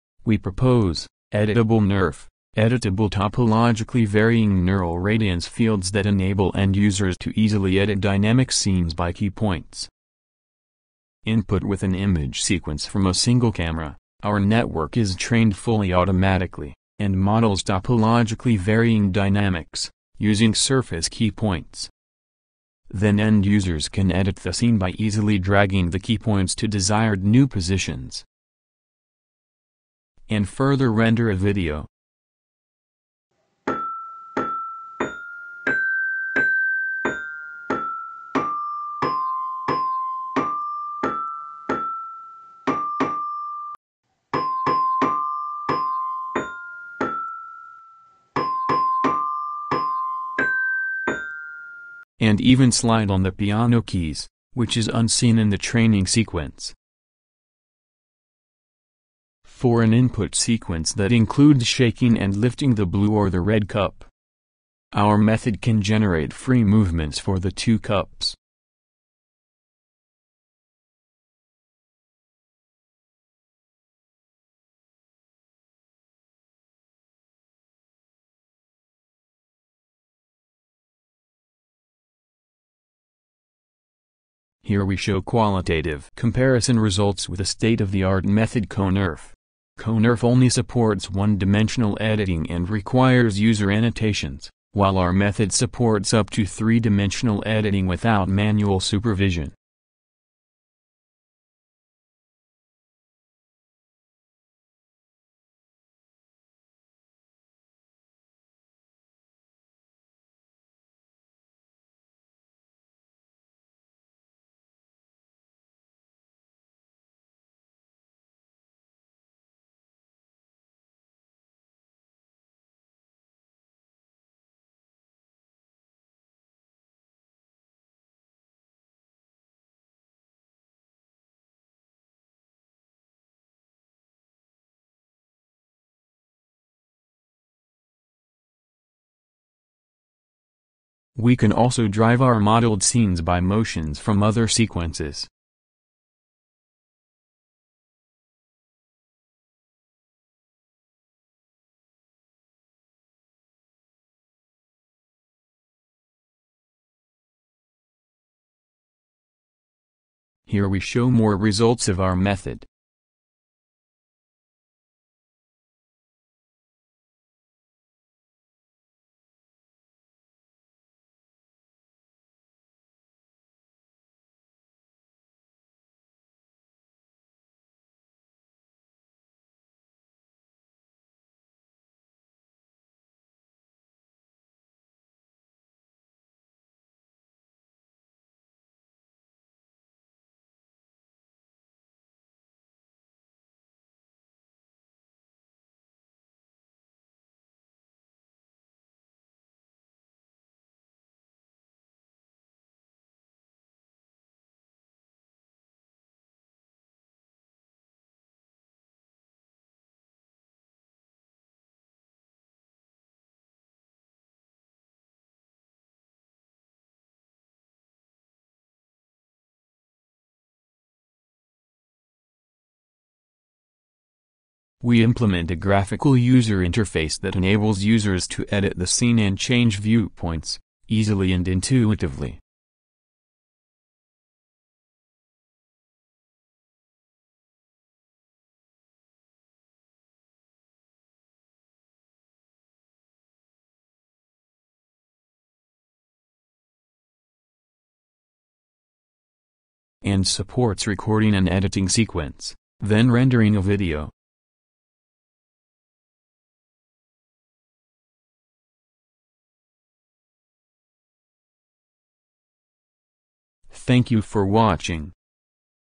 [CVPR 2023] EditableNeRF: Editing Topologically Varying Neural Radiance Fieldsby Key Points
Neural radiance fields (NeRF) achieve highly photo-realistic novel-view synthesis, but it's a challenging problem to edit the scenes modeled by NeRF-based methods, especially for dynamic scenes. We propose editable neural radiance fields that enable end-users to easily edit dynamic scenes and even support topological changes. Input with an image sequence from a single camera, our network is trained fully automatically and models topologically varying dynamics using our picked-out surface key points. Then end-users can edit the scene by easily dragging the key points to desired new positions. To achieve this, we propose a scene analysis method to detect and initialize key points by considering the dynamics in the scene, and a weighted key points strategy to model topologically varying dynamics by joint key points and weights optimization. Our method supports intuitive multi-dimensional (up to 3D) editing and can generate novel scenes that are unseen in the input sequence. Experiments demonstrate that our method achieves high-quality editing on various dynamic scenes and outperforms the state-of-the-art. Our code and captured data are available